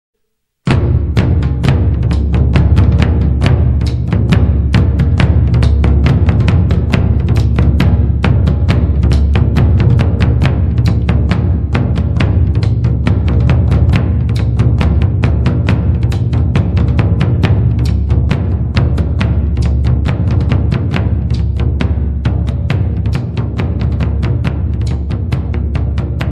saber 雙手刀--團體練習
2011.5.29圓明武學漢刀傳承班上課圖照集錦之團體練習

May 29,2011 Yuan Ming Traditional Martial Arts
Yuan Ming Han Dao Course Photos